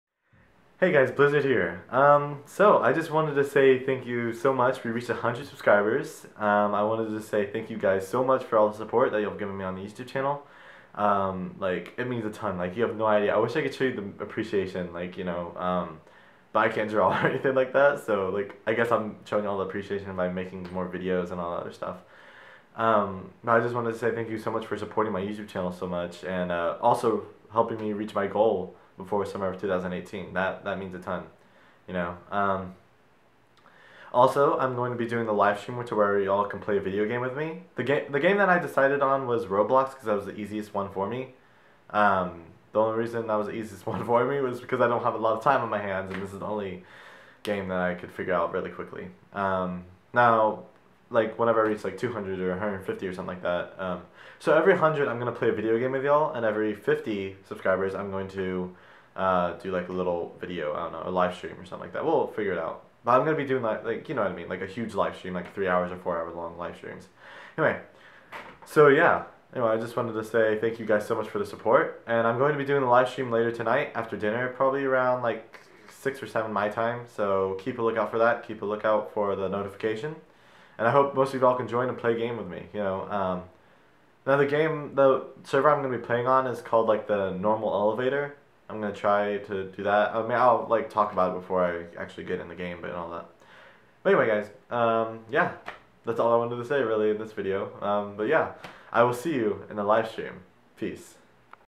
100 Subscribers video
Hey guys I just wanted to say thank you all so much for supporting my youtube channel so much it means a ton. Also I will be doing a live stream in a couple of hours so keep a lookout for that. Anyway guys I hope you enjoy the video!!!